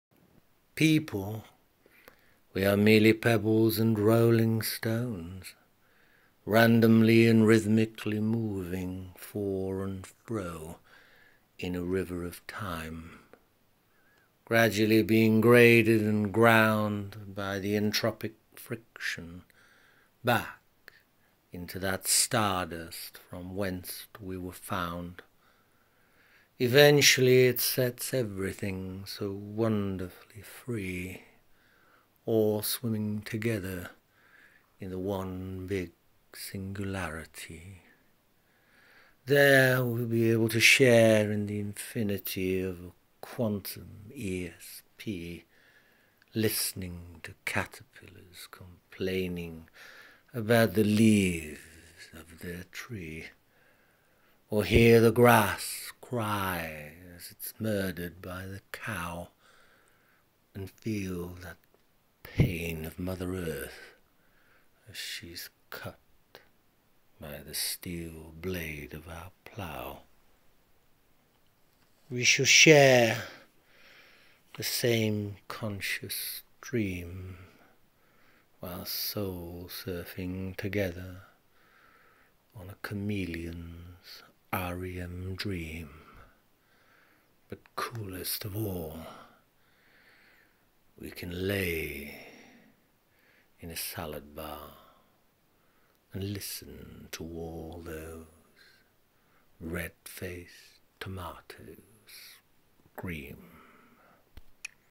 SCREAMING TOMATOES
An English king's breakfast could  consist  of swine flesh, embryos , fungi ,congealed blood , kippers, herring's, pike , roach various birds venison, cockles , pilchards .    when thinking about the Buddhist belief of  reincarnation  wouldn't it be a devine justice  if these gargantuan gluttonous  came back  as screaming red faced tomatoes in a vegan salad bar .